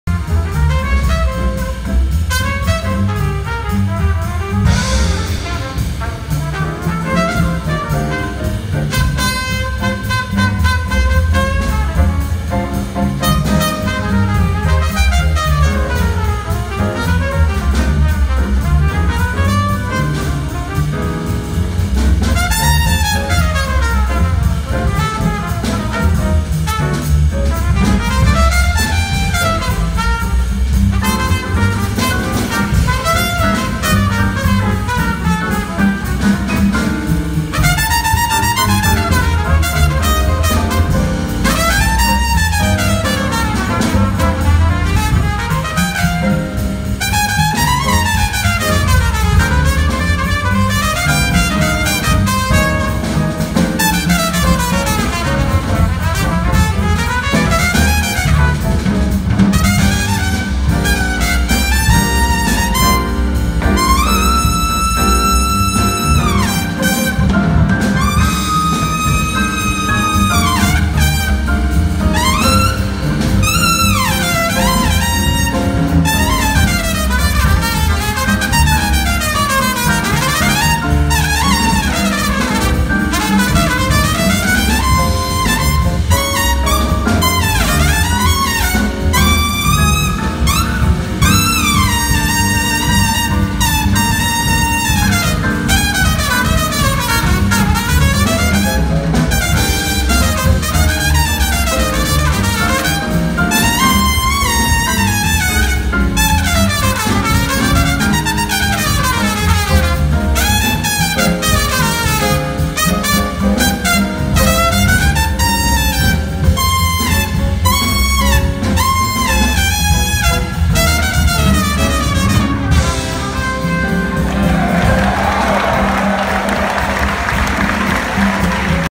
Terell Stafford Demolishes Trumpet
January 8, 2020. The day a trumpet was killed.